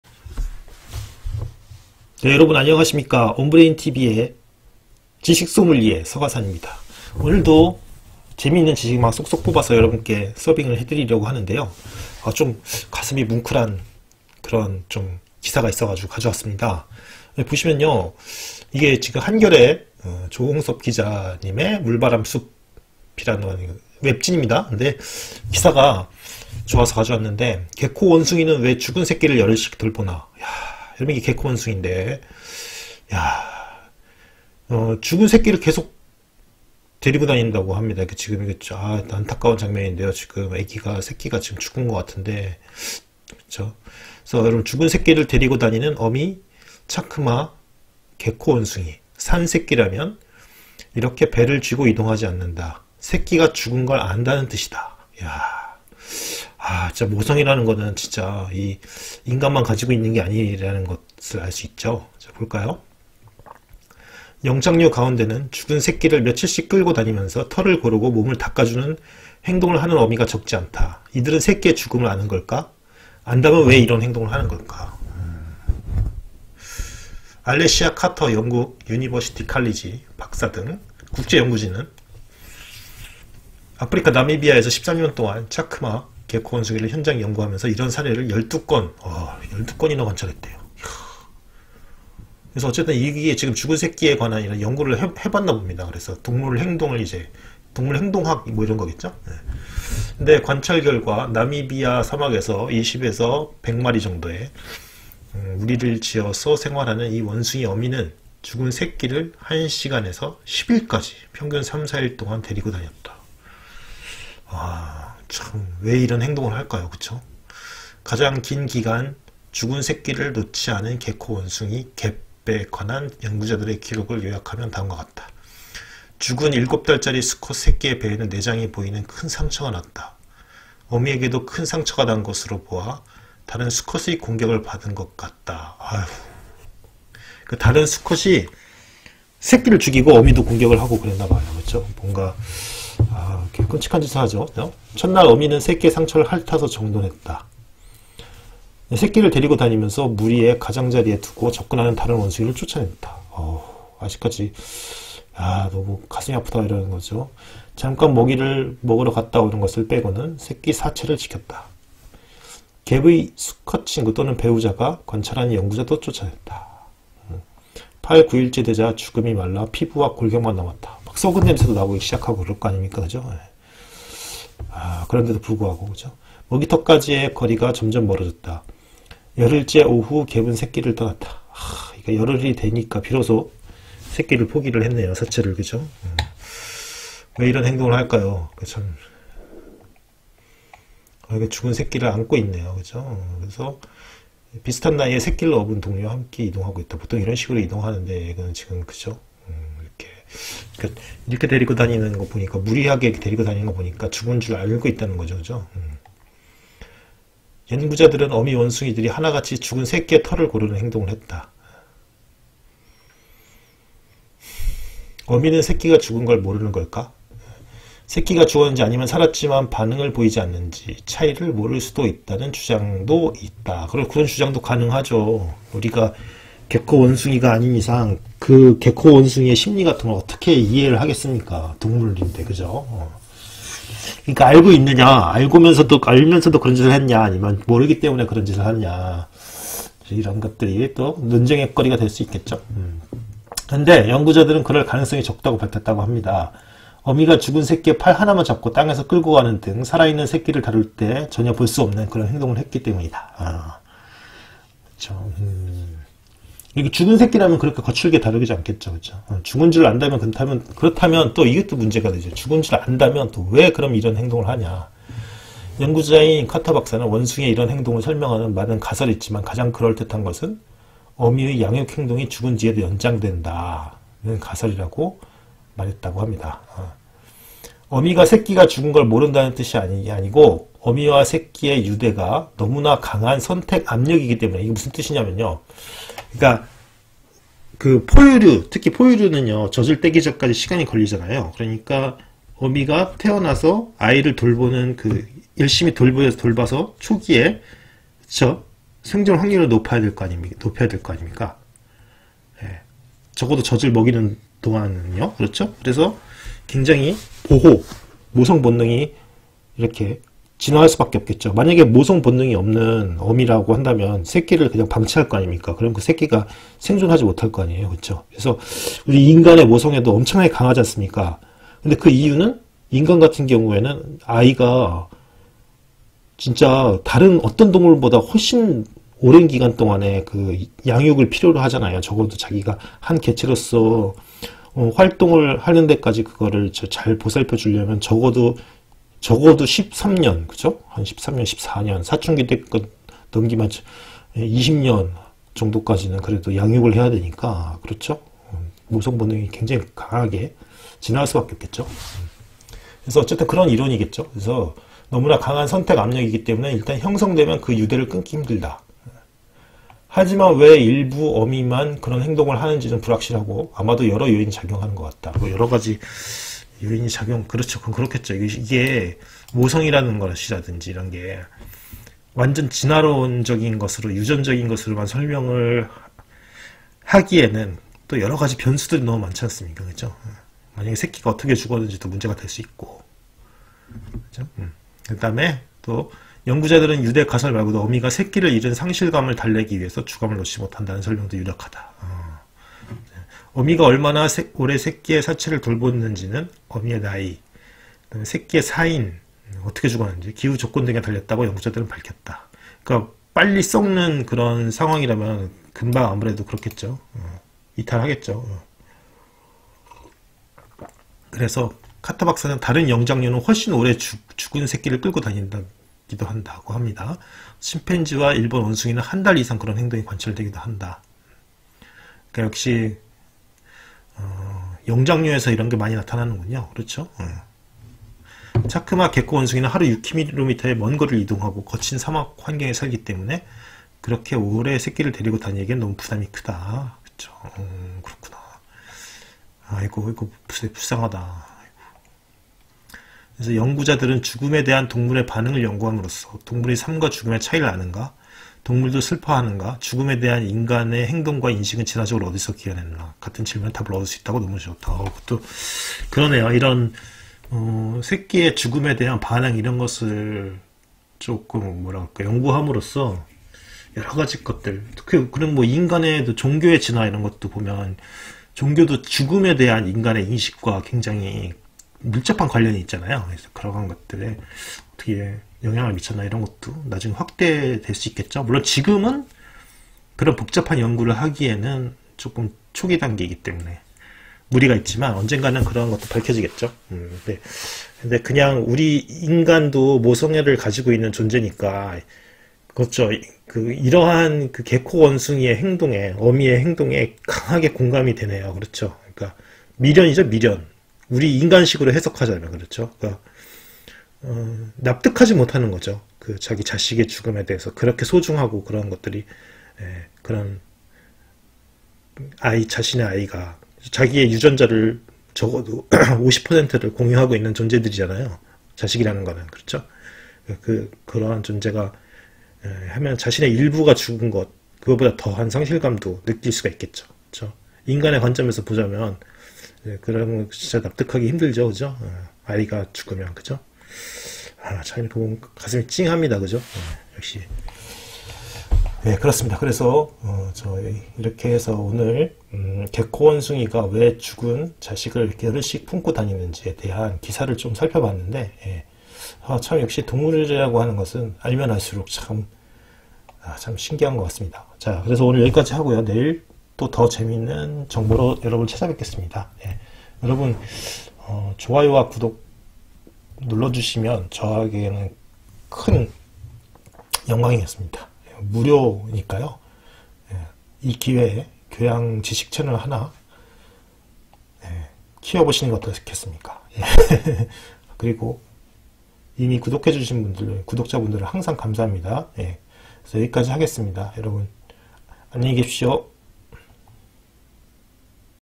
개코원숭이가 죽은 새끼를 열흘이나 안고 다니는 이유. 재밌는 교양 지식 백과
그 놀라운 모성을 확인해 봅시다!